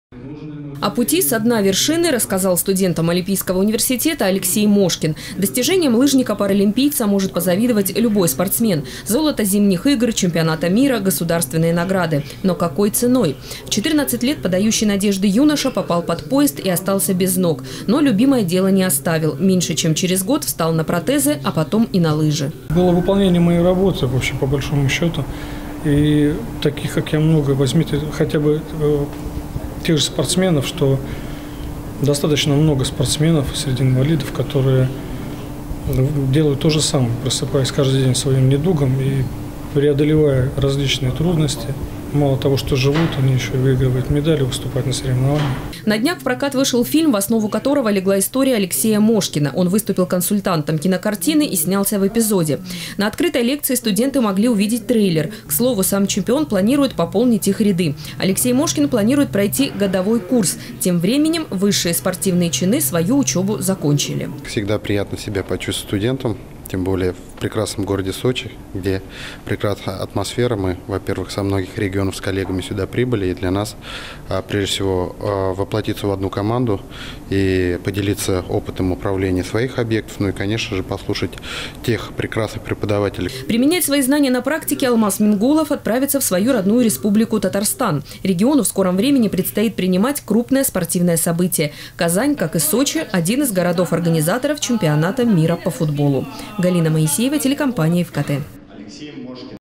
В РМОУ Сочи побывал двукратный чемпион Паралимпиады Алексей Мошкин. Новости Эфкате
Спортивную карьеру он закончил. Однако продолжает вдохновлять своей силой духа и спортсменов, и болельщиков.

Занимается общественной работой, консультирует режиссёров и внедряет в жизнь спортивную инклюзию. Обо всем этом Алексей Мошкин рассказал студентам олимпийского университета. О пути «Со дна вершины» рассказал студентам Олимпийского университета Алексей Мошкин. Достижениям лыжника-паралимпийца может позавидовать любой спортсмен. Золото Зимних игр, Чемпионата мира, государственные награды. Но какой ценой. В 14 лет подающий надежды юноша попал под поезд и остался без ног. Но любимое дело не оставил. Меньше чем через год встал на протезы, а потом и на лыжи.

Сочи. Новости.
Телеканал Эфкате.